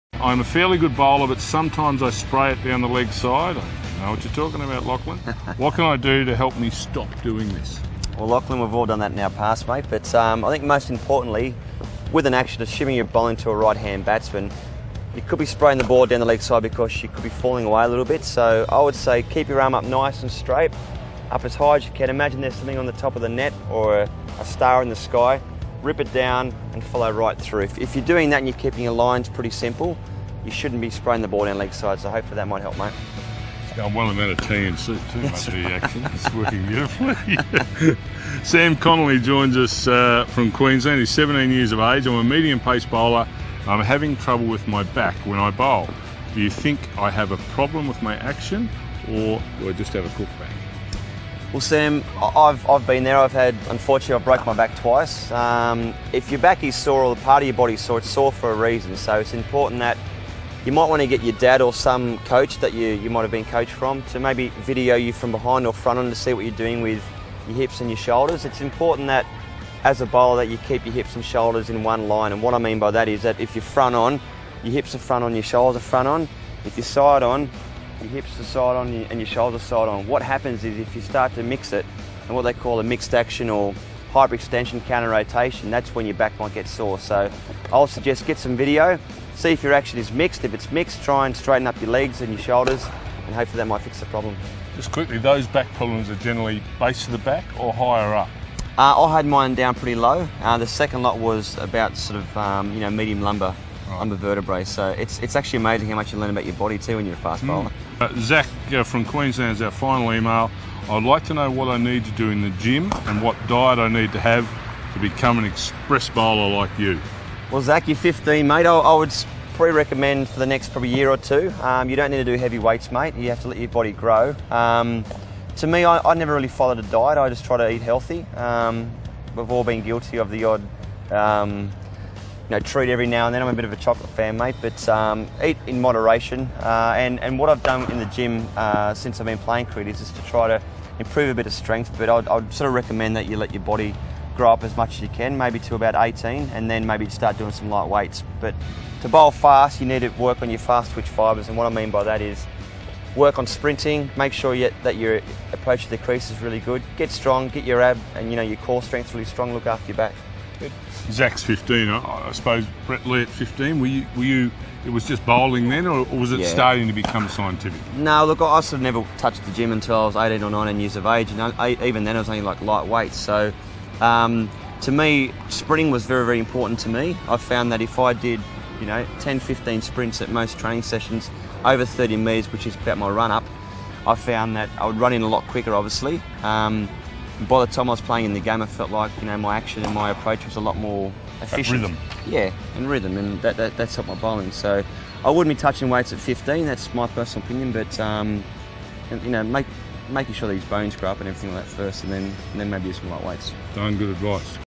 Fast Bowling Masterclass with Brett Lee - Great Bowling Tips
Masterclass Cricket With Speed Star Brett Lee
Brett Lee who was the fastest bowler in the world along with Shoaib Akhtar and Shaun Tait giving tips to the youngsters about fast bowling and various other techniques. Great Fast Bowling Tips
It is a great Fast Bowling Masterclass